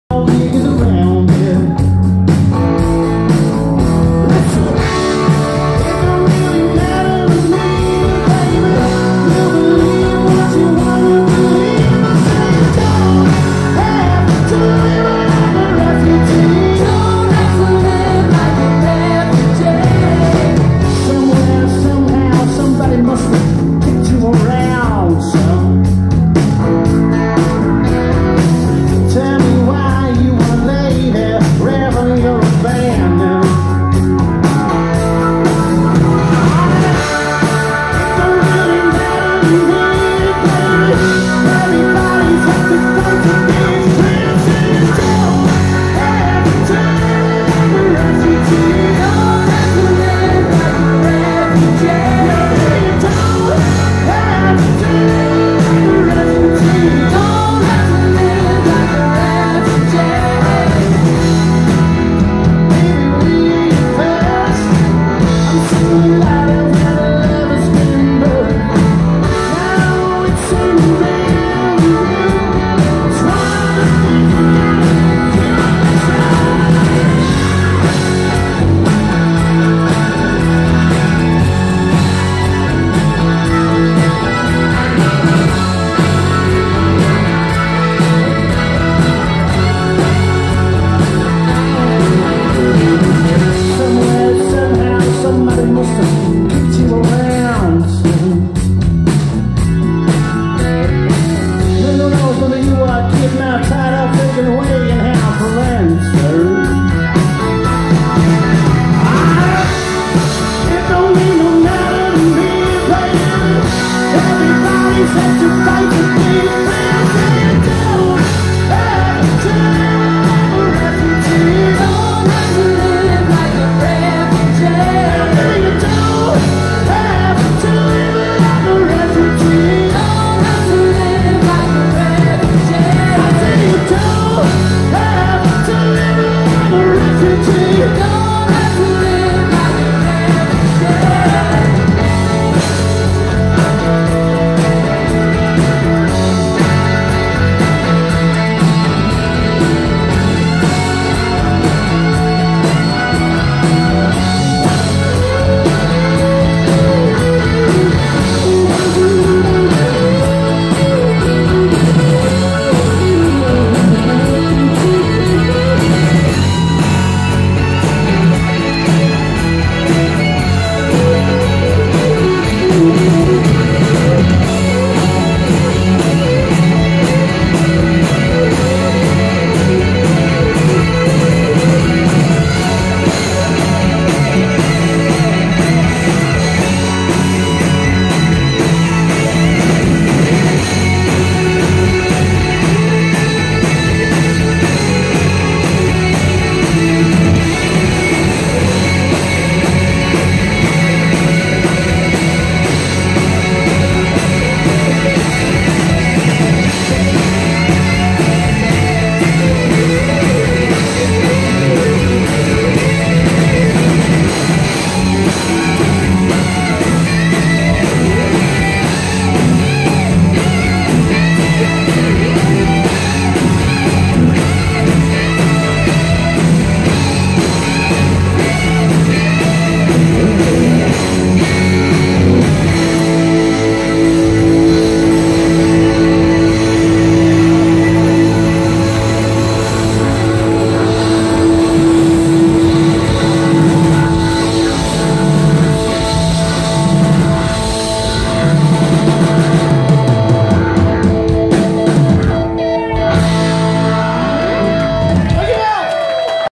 Voltage plays Tom Petty , Bibelot , 19-01-2019
Dave Vermeulen en de band Voltage spelen Tom Petty in Bibelot, Dordrecht, 19 januari 2019.
Prima optreden voor een select groepje liefhebbers.